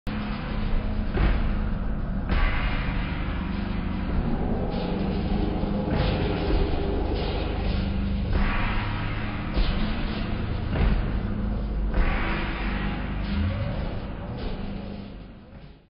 Stage dive slowmo from yesterdays show with Refused
Now this is what I call a stage dive!